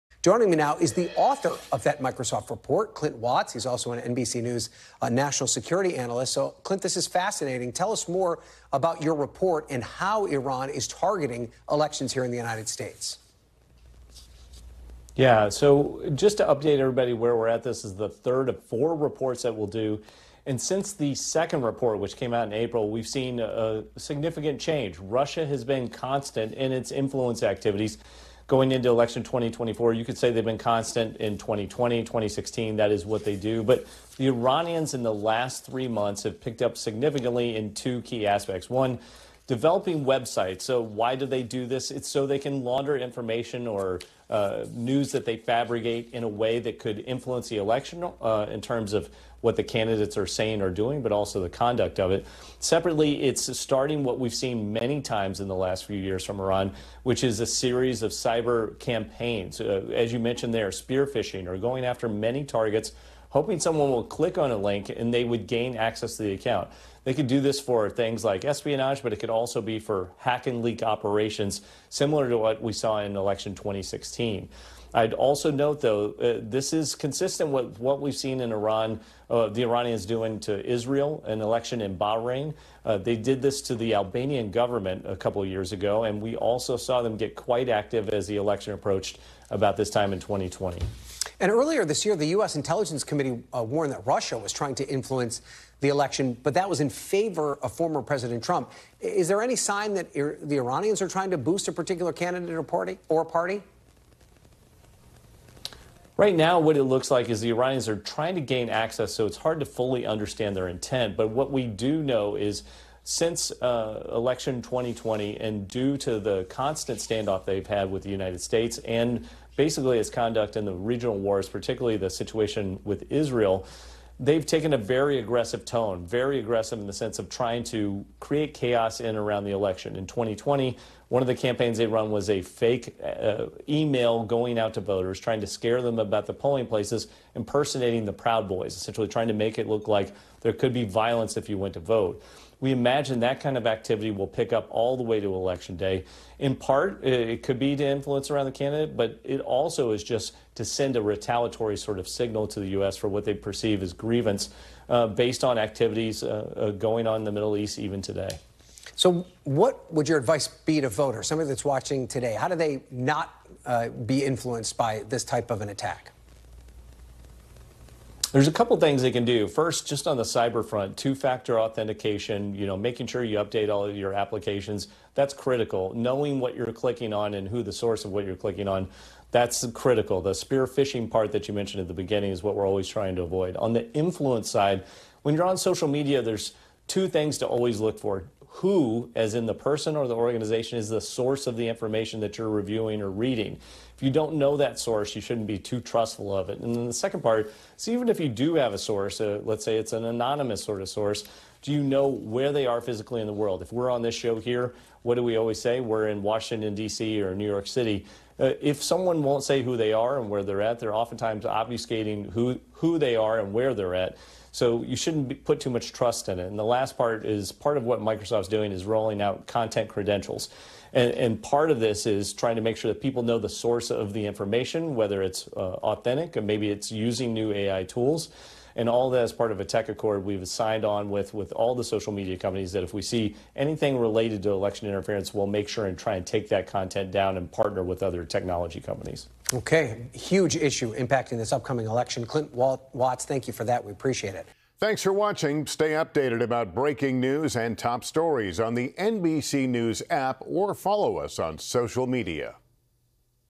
Iran is increasing cyber operations to try to ‘create chaos’ around U.S. elections, Microsoft warns
In a new report, Microsoft is warning about Iran’s covert cyber operations aimed at influencing the U.S. elections. Clint Watts, the author of that report and NBC News national security analyst, explains.